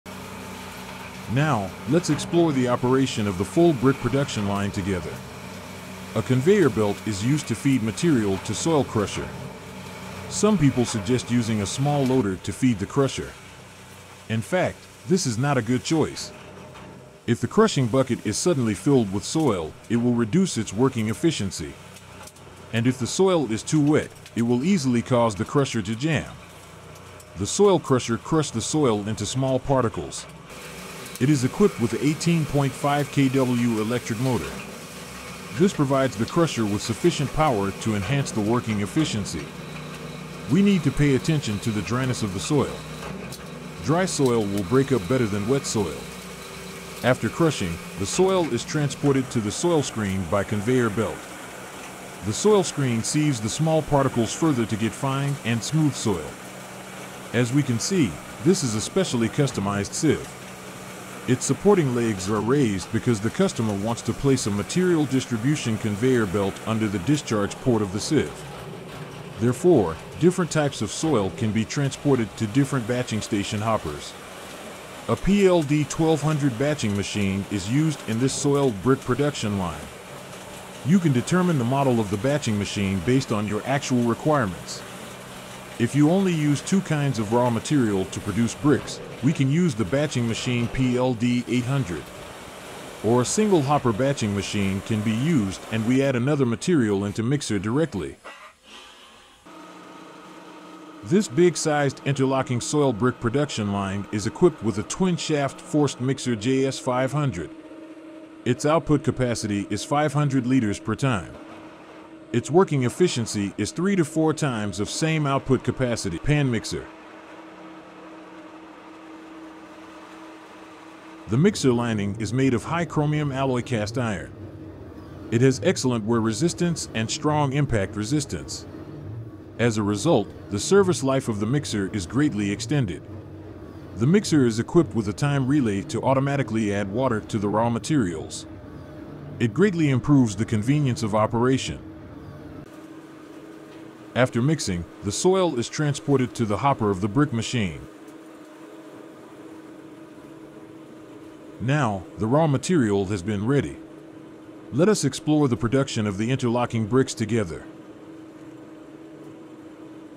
How to Process Raw Materials to Produce Interlocking Soil Bricks CSEB
This video shows the process of processing raw materials in interlocking earth bricks production.
First, the crusher to break the earth into small particles.
Secondly, the earth is further refined through a sieve.
Thirdly, the batching machine weigh raw material based a certain ratio.
Fourthly, all the raw materials are mixed evenly in the mixer.
Finally, the raw materials are conveyored to the storage hopper of the brick making machine.